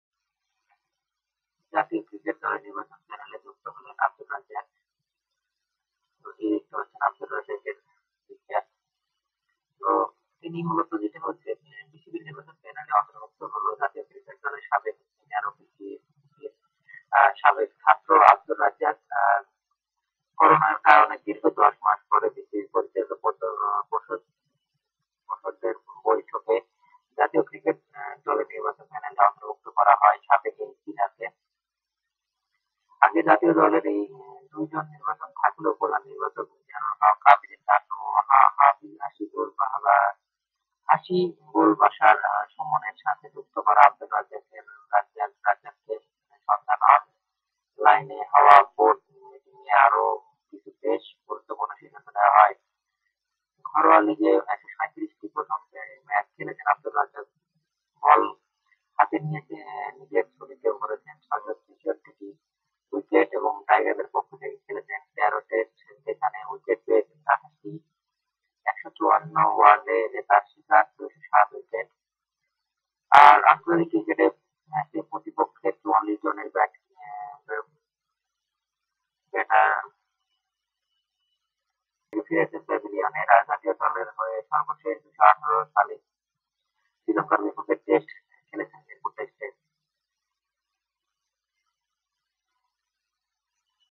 জাতীয় ক্রিকেট দলের নির্বাচক প্যানেলে যুক্ত হলেন আব্দুর রাজ্জাক রাজ
বিসিবির নির্বাচক প্যানেলে অন্তুভুক্ত হলো জাতীয় ক্রিকেট দলের সাবেক স্পিনার ও বিকেএসপির সাবেক ছাত্র আব্দুর রাজ্জাক রাজ। করোনার কারণে দীর্ঘ দশ মাস পরে বিসিবির পরিচালক পর্ষদের বৈঠকে জাতীয় ক্রিকেট দলের নির্বাচক প্যানেলে অন্তুভুক্ত করা হয় সাবেক এই স্পিনারকে।

আগে জাতীয় দলের দুই জন নির্বাচক থাকলেও প্রধান নির্বাচক মিনহাজুল আবেদিন নান্নু ও হাসিবুল বাশার সুমনের সাথে যুক্ত করা আব্দুর রাজ্জাক রাজকে। সন্ধ্যায় অনলাইনে হওয়া বোর্ড মিটিংয়ে আরও কিছু বেশ গুরুত্বপূর্ণ সিদ্ধান্ত নেয়া হয়।

ঘরোয়া লিগে ১৩৭টি প্রথম শ্রেণির ম্যাচ খেলেছেন রাজ্জাক, বল হাতে নিজের ঝুলিতে ভরেছেন ৬৪৩টি উইকেট। টাইগারদের পক্ষে খেলেছেন ১৩ টেস্ট যেখানে উইকেট পেয়েছেন ২৮টি।

১৫৩ ওয়ানডেতে তার শিকার ২০৭ উইকেট। আর আন্তর্জাতিক টি-টোয়েন্টি ম্যাচে প্রতিপক্ষের ৪৪ জন ব্যাটারকে ফিরিয়েছেন প্যাভিলিয়নে। জাতীয় দলের হয়ে সর্বশেষ ২০১৮ সালে শ্রীলঙ্কার বিপক্ষে টেস্ট খেলেছিলেন মিরপুর টেস্টে।

Laptop
My Laptop
Microphone
My Mic